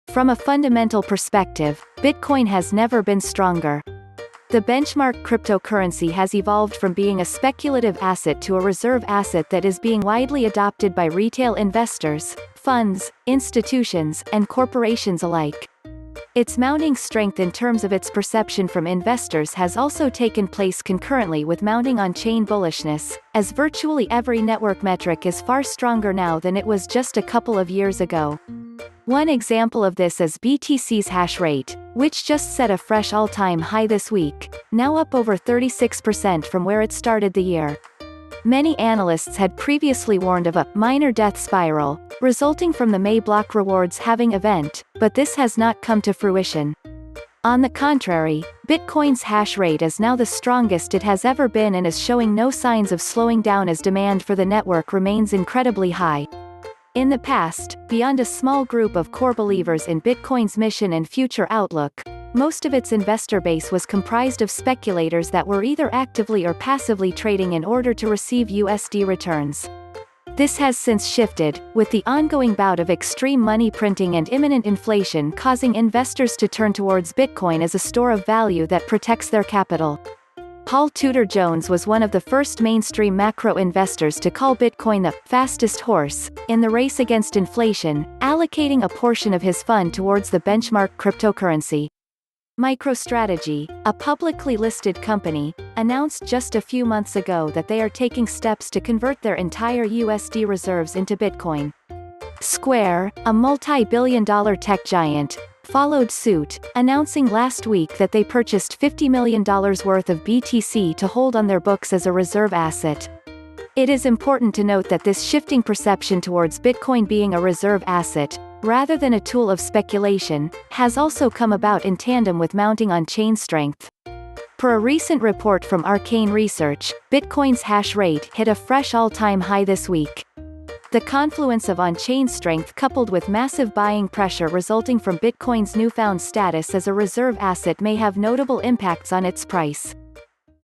Bitcoin Hash Rate Rockets to New All Time High; Up 36% in 2020
From a fundamental perspective, Bitcoin has never been stronger. The benchmark cryptocurrency has evolved from being a speculative asset to a reserve asset that is being widely adopted by retail investors, funds, institutions, and corporations alike. Its mounting strength in terms of its perception from investors has also taken place concurrently with mounting on-chain bullishness, as virtually every network metric is far stronger now than it was just a couple of years ago. One example of this is BTC’s hash rate, which just set a fresh all-time high this week, now up over 36% from where it started the year. Many analysts had previously warned of a “miner death spiral” resulting from the May block rewards halving event, but this has not come to fruition. On the contrary, Bitcoin’s hash rate is now the strongest it has ever been and is showing no signs of slowing down as demand for the network remains incredibly high. In the past, beyond a small group of core believers in Bitcoin’s mission and future outlook, most of its investor base was comprised of speculators that were either actively or passively trading in order to receive USD returns. This has since shifted, with the ongoing bout of extreme money printing and imminent inflation causing investors to turn towards Bitcoin as a store of value that protects their capital. Paul Tudor Jones was one of the first mainstream macro investors to call Bitcoin the “fastest horse” in the race against inflation, allocating a portion of his fund towards the benchmark cryptocurrency. MicroStrategy – a publicly listed company – announced just a few months ago that they are taking steps to convert their entire USD reserves into Bitcoin. Square – a multi-billion-dollar tech giant – followed suit, announcing last week that they purchased $50 million worth of BTC to hold on their books as a reserve asset. It is important to note that this shifting perception towards Bitcoin being a reserve asset – rather than a tool of speculation – has also come about in tandem with mounting on-chain strength. Per a recent report from Arcane Research, Bitcoin’s hash rate hit a fresh all-time high this week. The confluence of on-chain strength coupled with massive buying pressure resulting from Bitcoin’s newfound status as a reserve asset may have notable impacts on its price.